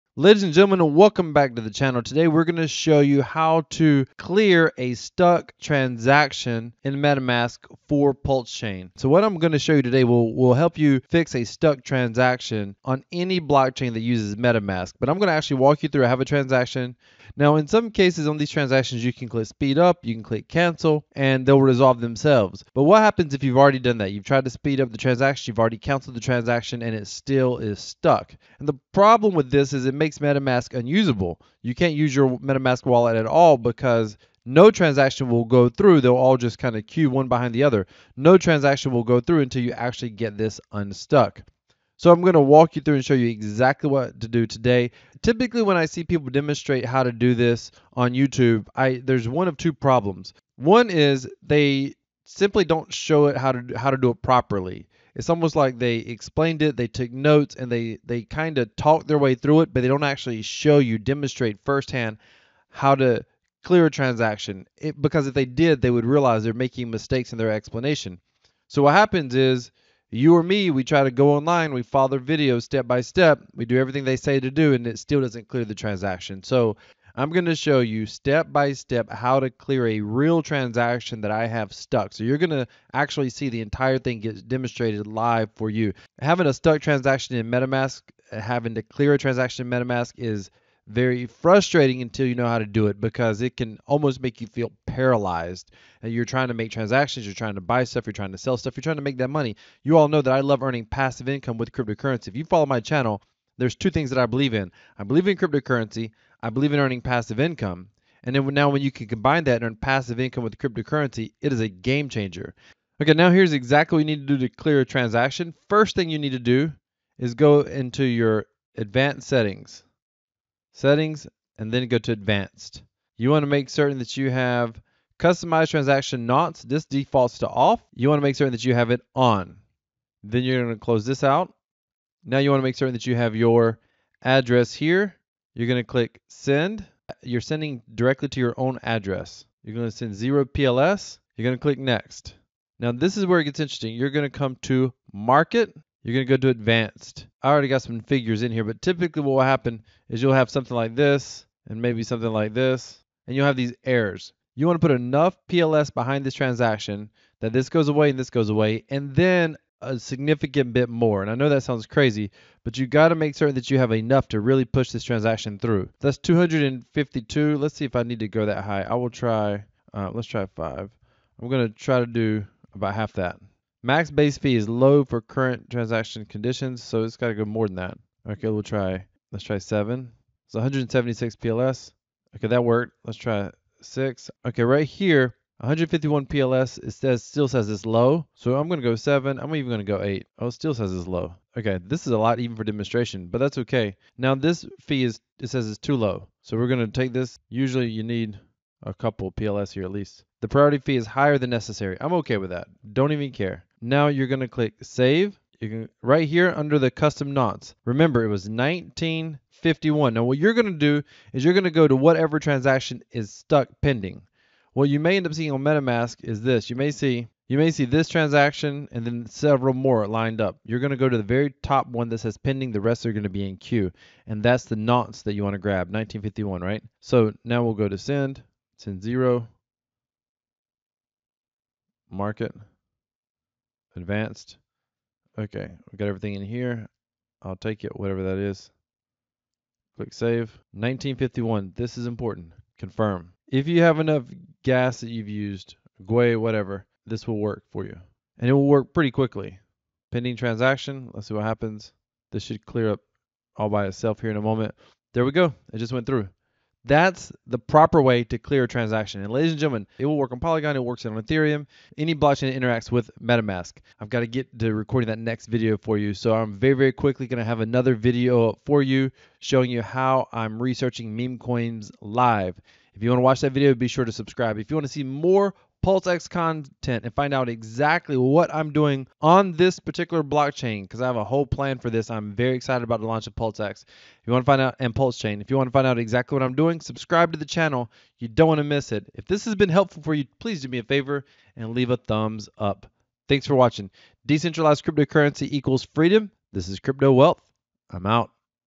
How to Fix a Stuck Transaction on (Pulsechain) Metamask
In this video I demonstrate how I fix a stuck transaction on metamask while using the Pulsechain. Watch my video: How to Pick the Next 100x Meme Coin

Do not consider my content as anything other than Satire. This is for your entertainment. Not investing advice.